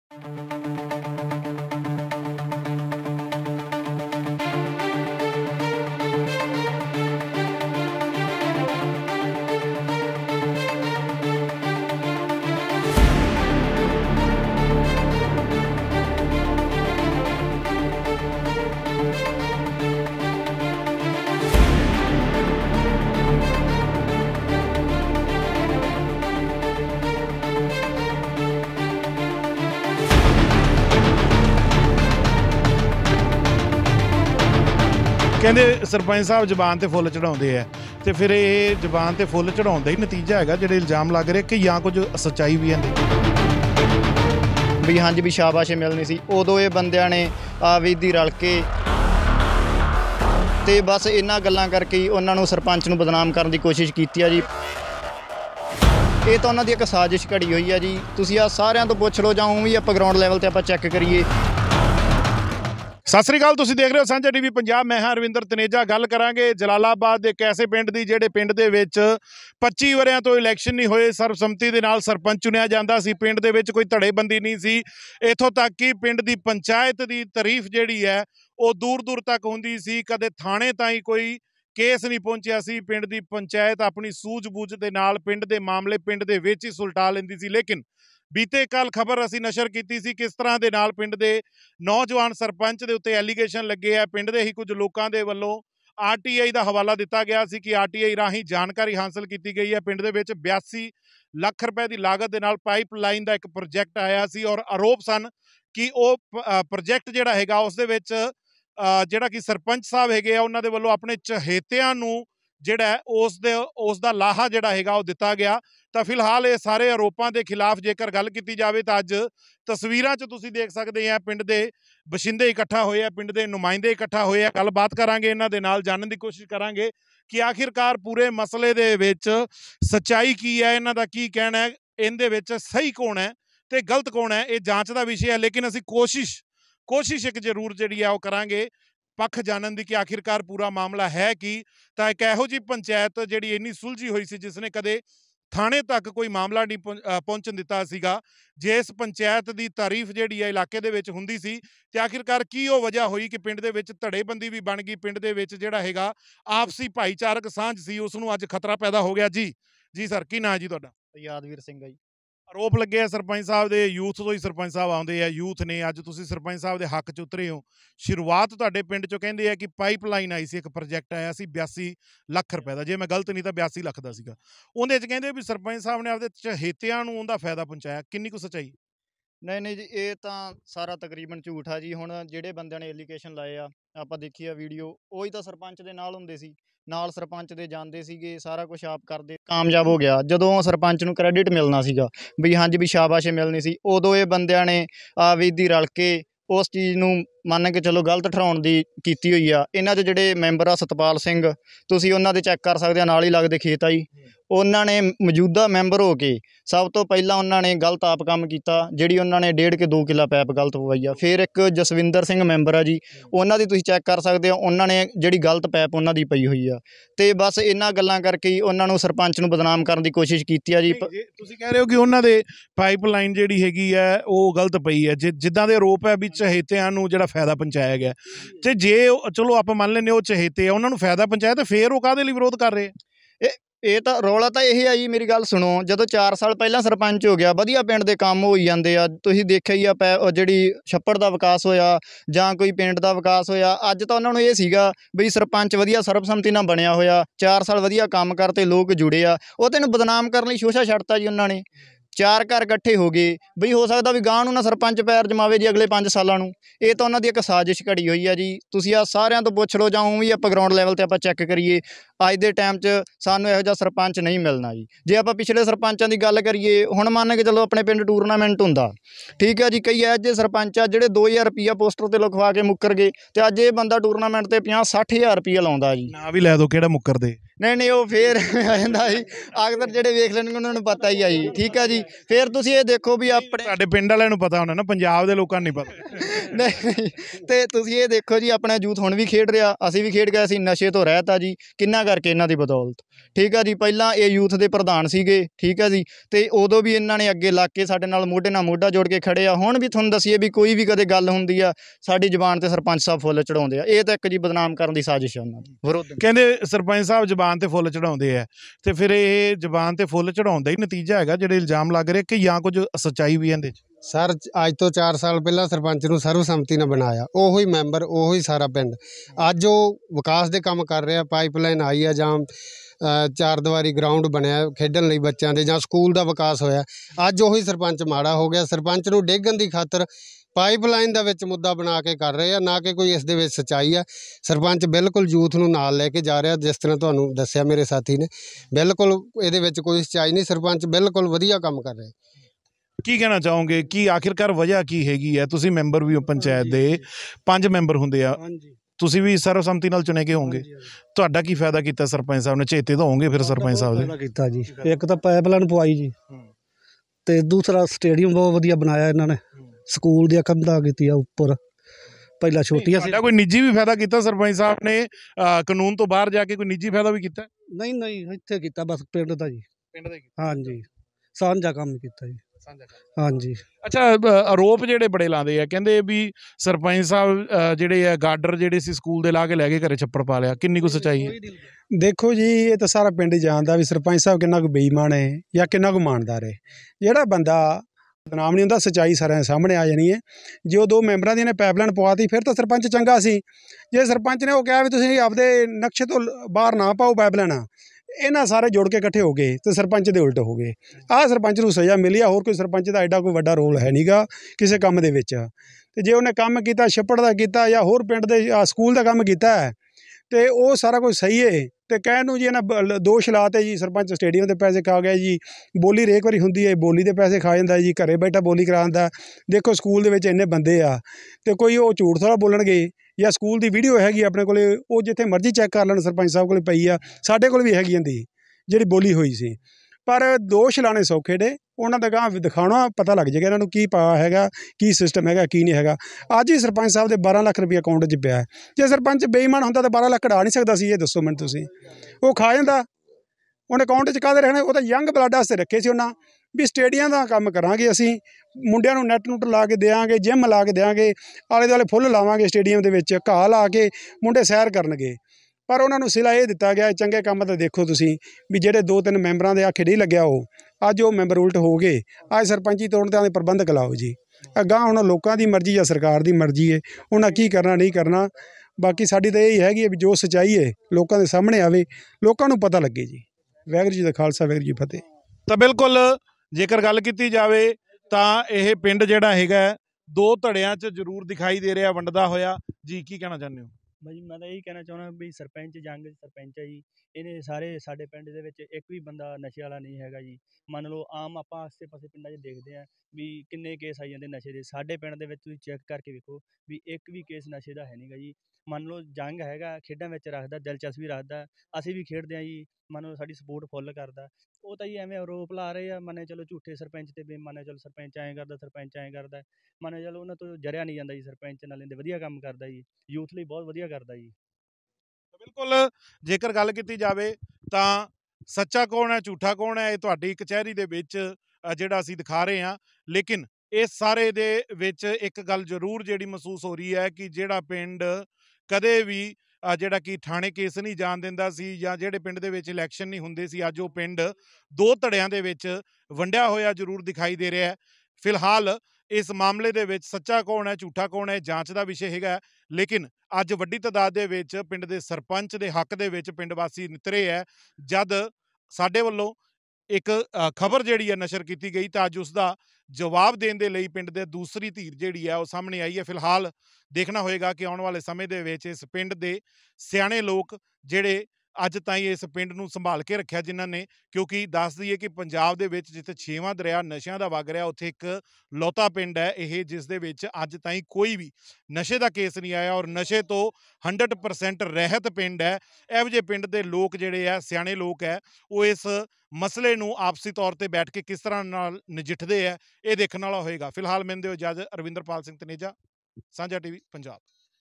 ਜਲਾਲਾਬਾਦ ਪਿੰਡ ਕੱਚਾ ਕਾਲੇ ਵਾਲਾ ਸਰਪੰਚ ਦੇ ਹੱਕ ‘ਚ ਨਿੱਤਰੇ ਪਿੰਡ ਵਾਸੀ
ਮੌਜੂਦਾ ਸਰਪੰਚ ਨੂੰ ਬਦਨਾਮ ਕਰਨ ਦੀ ਦੱਸਿਆ ਸਾਜਿਸ਼
VMedia # 806

Sling TV

Watcho-d2hApp
mxplayer